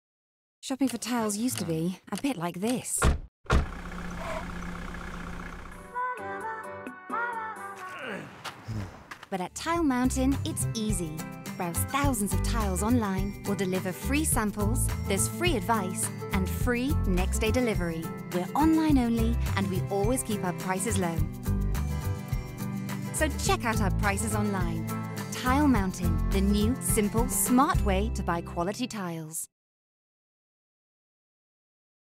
Tile Mountain TV Ad 2014
Tile Mountain makes its small screen debut with its very own TV ad, first shown in 2014 across several UK commercial TV channels. As you'll see from the trailer, there's no simpler way to buy quality tiles, and thanks to our free sample service that allows you to have cut samples of the products you're considering delivered straight to your door, you can really get a feel for your chosen tiles before making a purchase.

See what your chosen tiles will look like with our visualiser